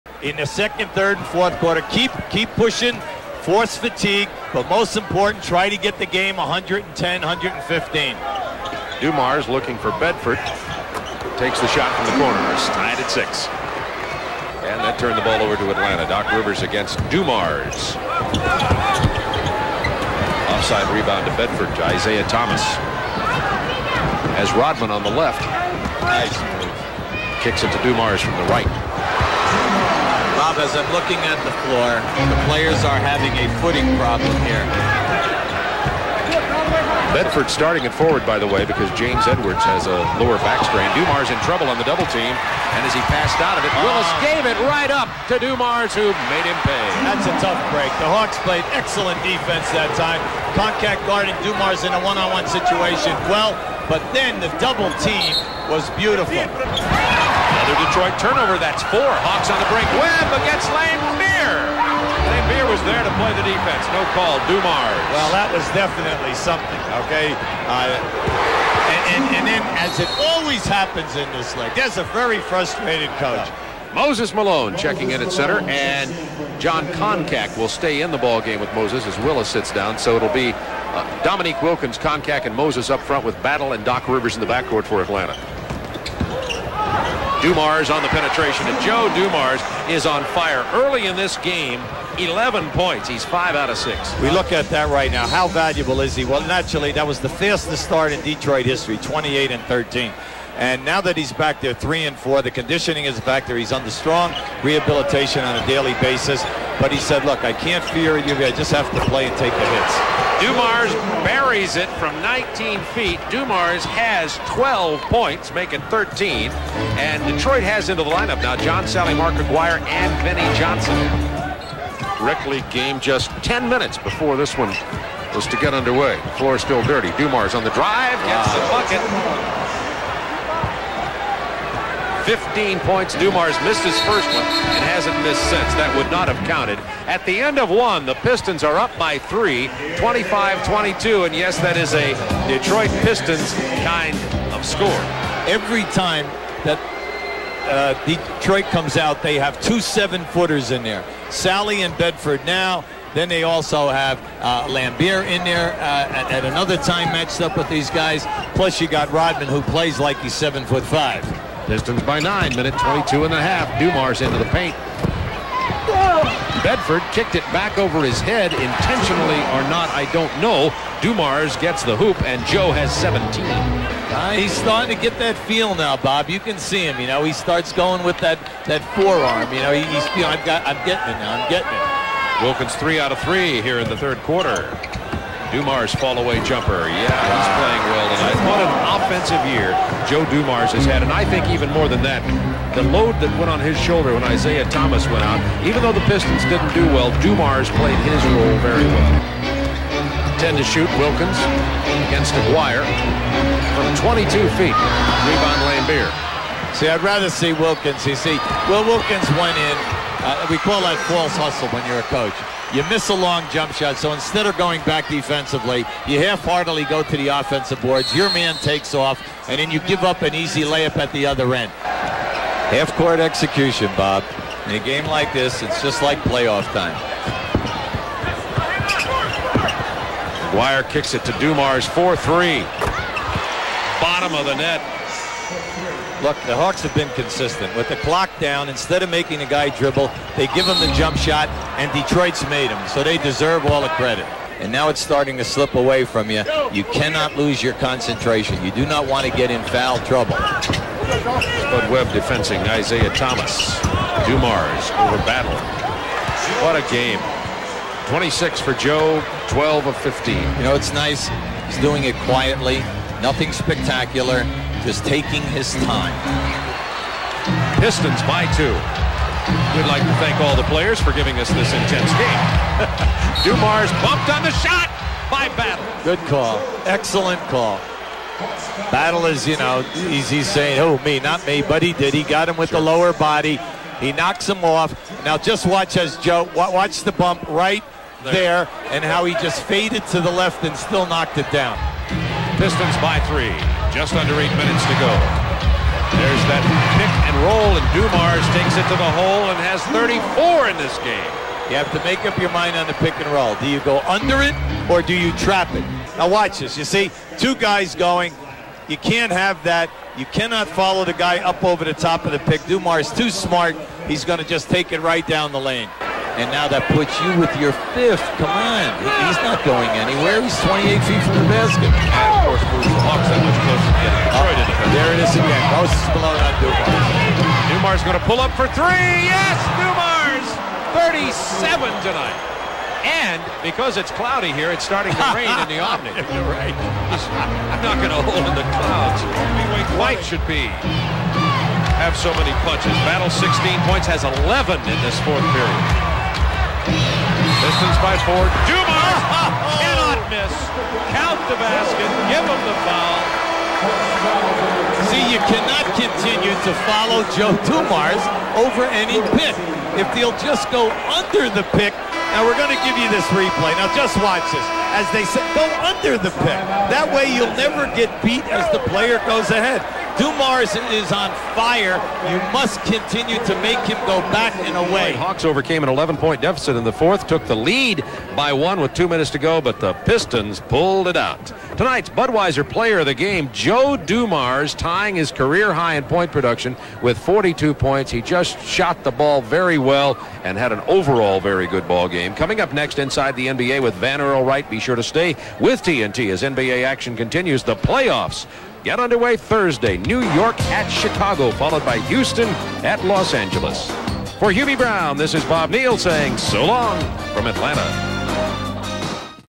Joe Dumars Sizzles, Drops 42 on the Hawks (1991)
April 19, 1991 - Detroit Pistons guard Joe Dumars matched a then-career-high by scoring 42 points in a 126-120 win over the Atlanta Hawks in the penultimate game of the 1990-91 regular season. Dumars shot 16-of-23 from the field in all, hitting both of his 3-point attempts, and connecting on 8-of-10 from the line. His high water mark would be re-established one season later, when he laid a 45-spot upon the shoulders of the Golden State Warriors.

Special thanks to Hardwood and Hoops